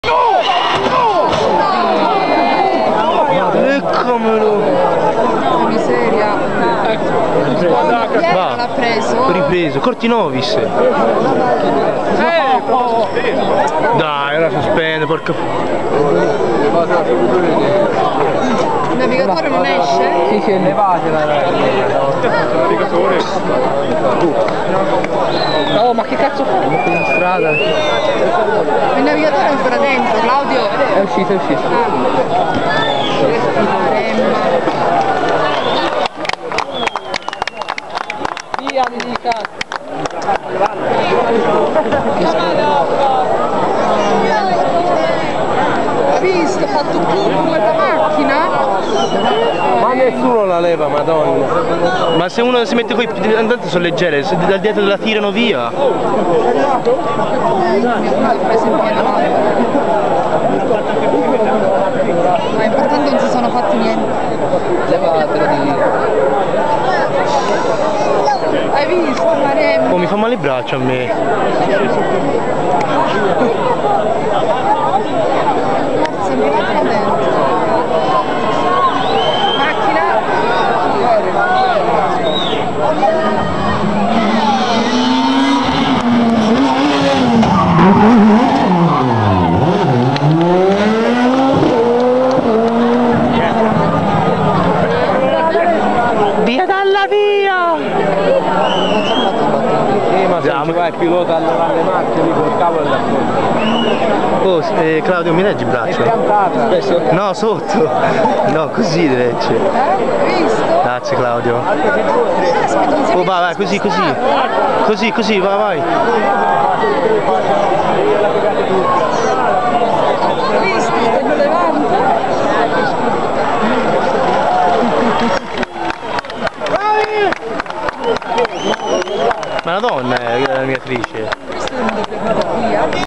Incidente Cortinovis al Rally degli Abeti 2010
brutto incidente di cortinovis sulla 2°ps lizzano!
per fortuna l'equipaggio ne esce senza conseguenza